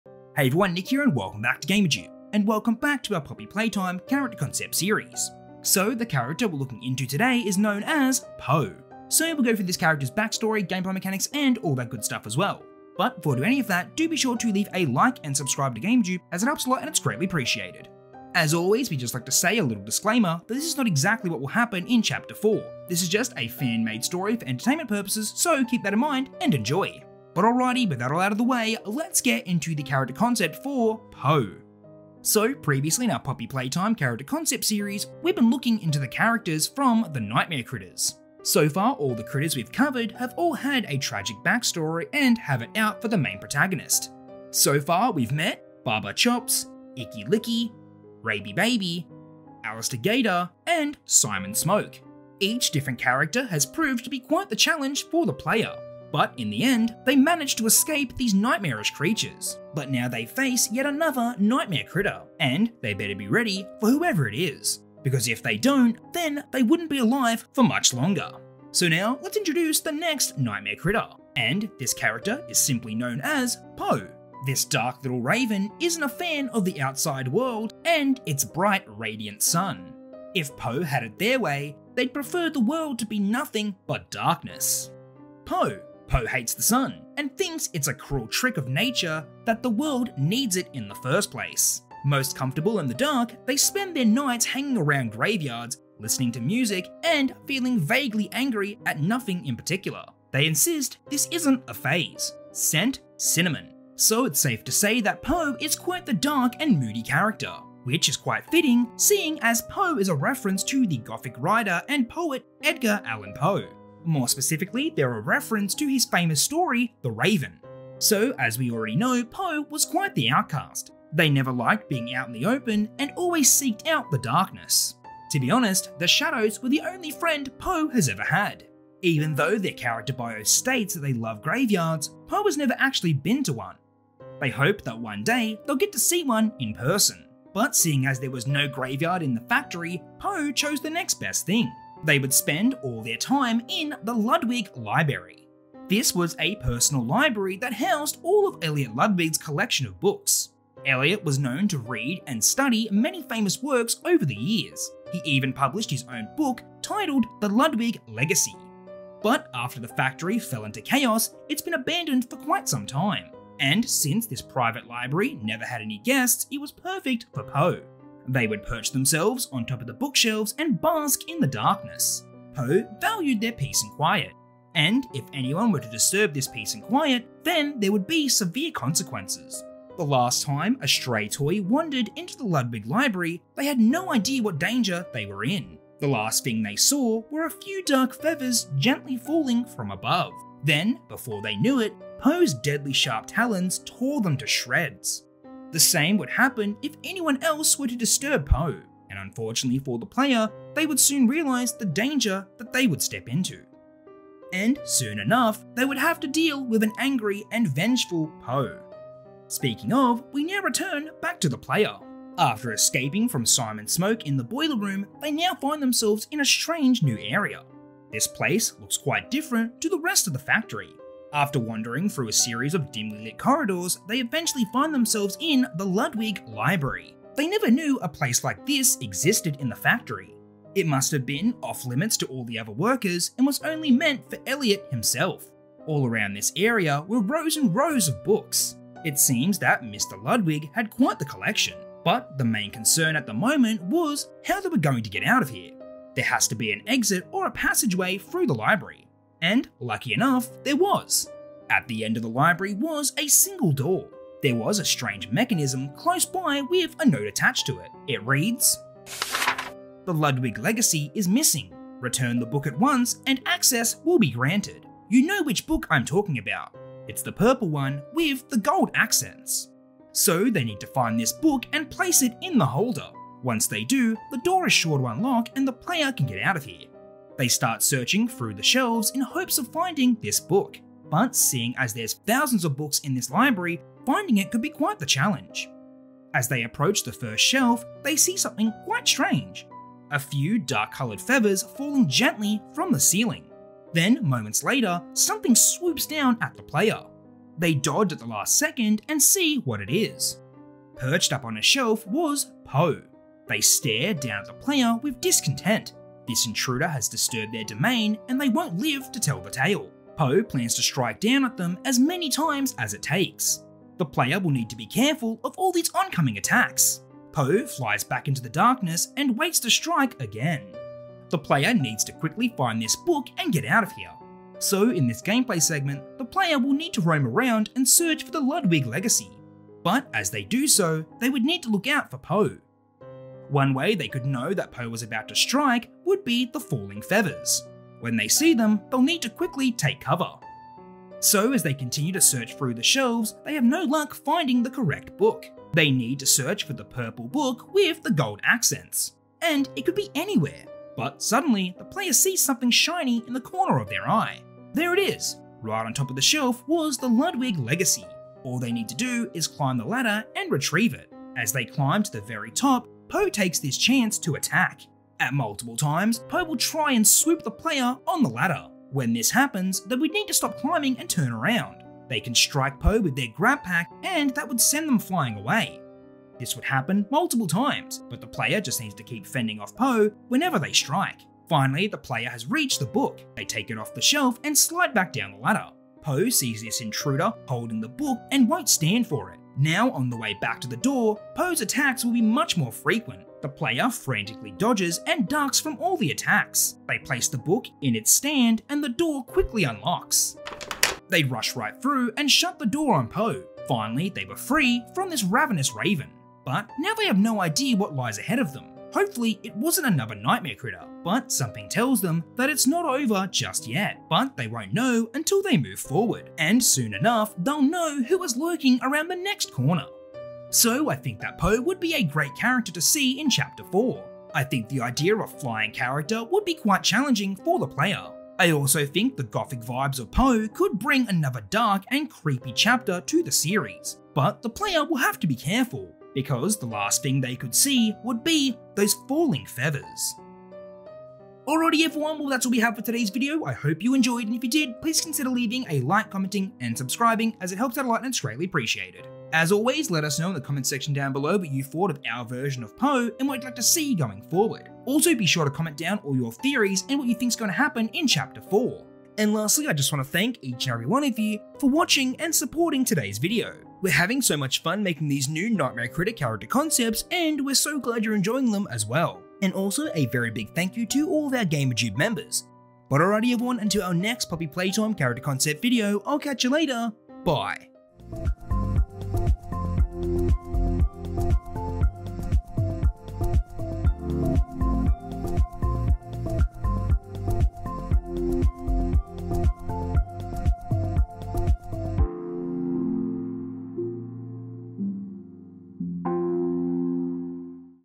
What Could Be In Poppy Playtime | Poe | Nightmare Critters | Character Concept -Chapter 4
In todays video I talk about an idea for a Poppy Playtime concept character:  Poe

In this video we look into Poe and also the area that they reside in. We take look at their creepy Toy Factory, their back story and also how to run and hide from this creepy toy

and tell us down what you think and what you'd like to see going forward with the Poppy Playtime series.

Poppy Playtime.  In-Game. You must stay alive in this horror/puzzle adventure. Try to survive the vengeful toys waiting for you in the abandoned toy factory. Use your GrabPack to hack electrical circuits or nab anything from afar

Poppy Playtime published by MOB Games
Developed by MOB Games